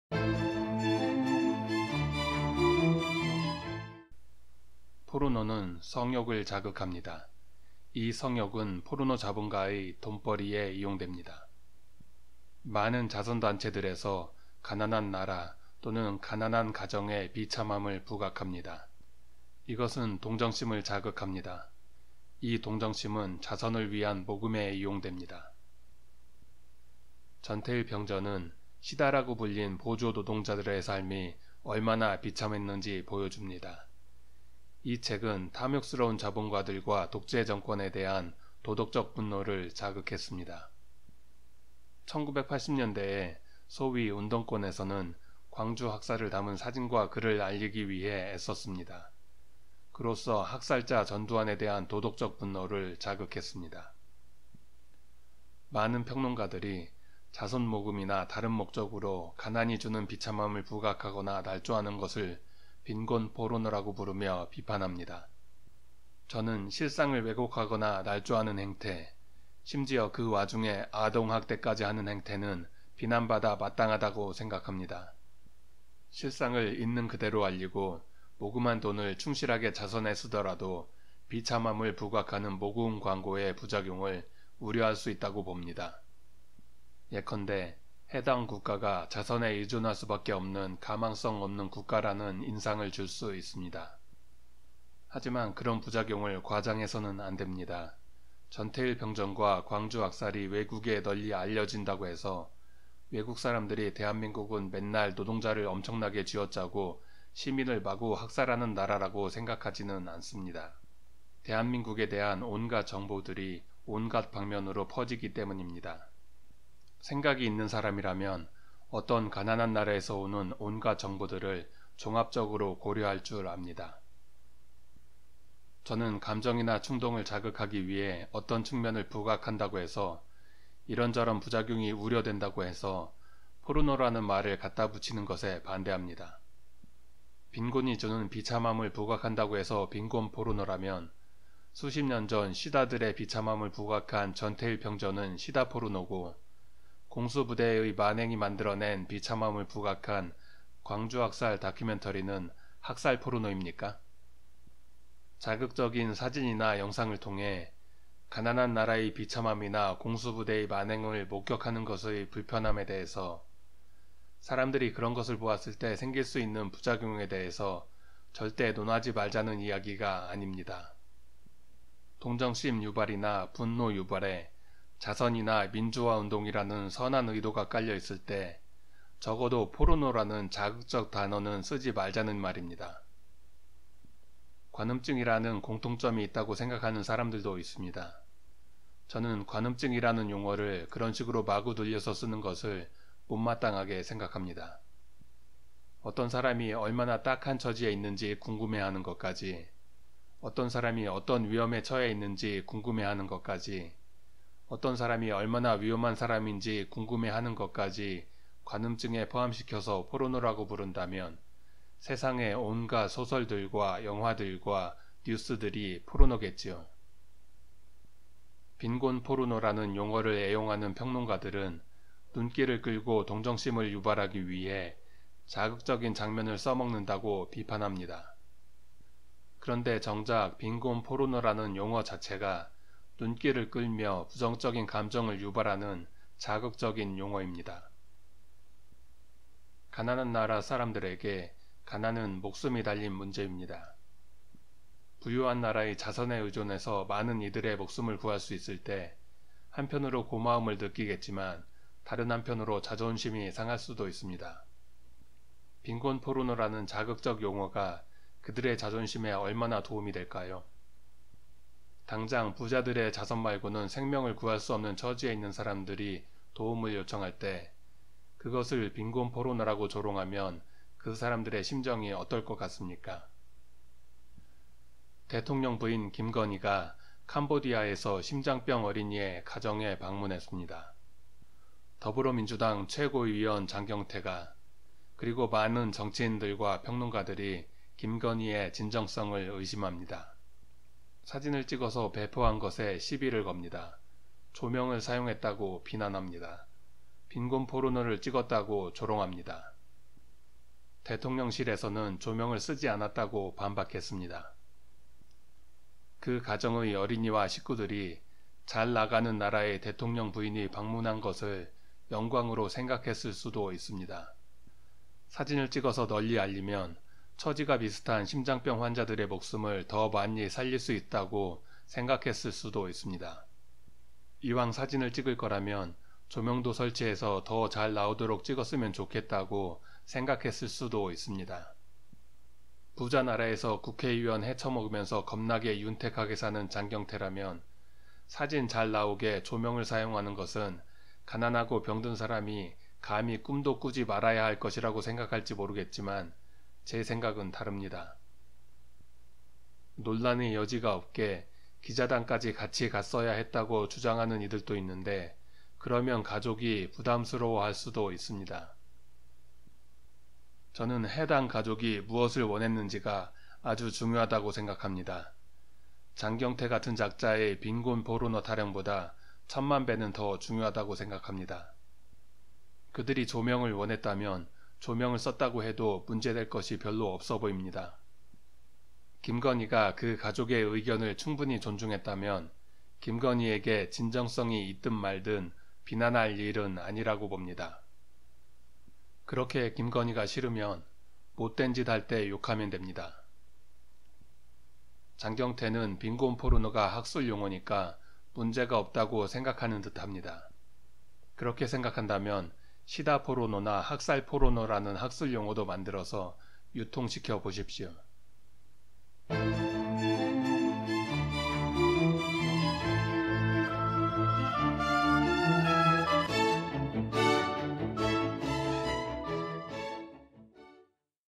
이덕하의 정치사회 평론 01. 빈곤 포르노라는 용어 자체가 가난한 사람들에 대한 배려 부족이다 (전태일 평전은 시다 포르노고, 광주 학살 다큐는 학살 포르노냐?)
대통령 부인 김건희가 캄보디아의 심장병 어린이의 가정에 방문해서 사진을 찍은 것을 두고 더불어민주당 최고위원 장경태가 빈곤 포르노라며 비난했습니다. 저는 포르노라는 단어를 이렇게 마구 붙이는 것에 반대합니다. 그리고 해당 가족이 원하는 것이 장경태의 빈곤 포르노 타령보다 훨씬 더 중요하다고 봅니다.